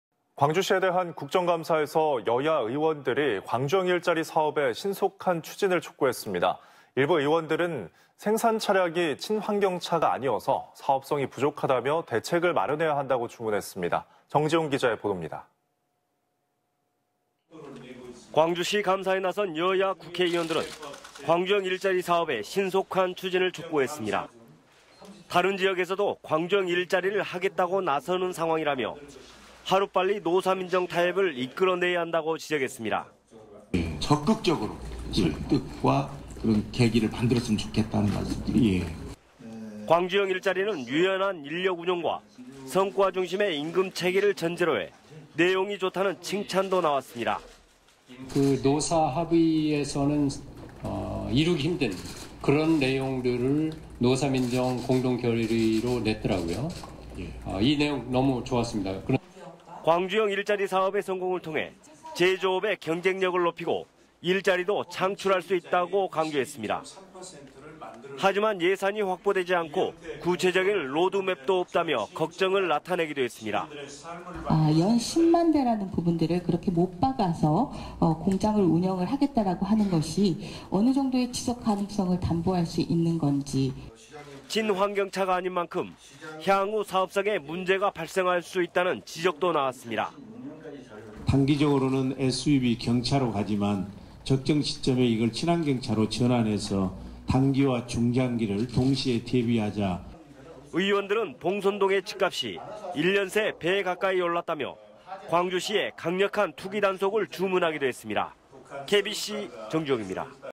[국감 현장] 광주형 일자리 신속 추진 '한 목소리'
광주시에 대한 국정감사에서 여야 의원들이 '광주형일자리' 사업의 신속한 추진을 촉구했습니다.

일부 의원은 생산차량이 친환경차가 아니어서 사업성이 부족하다며 대책을 마련해야 한다고 주문했습니다.

정지용 기자의 보돕니다.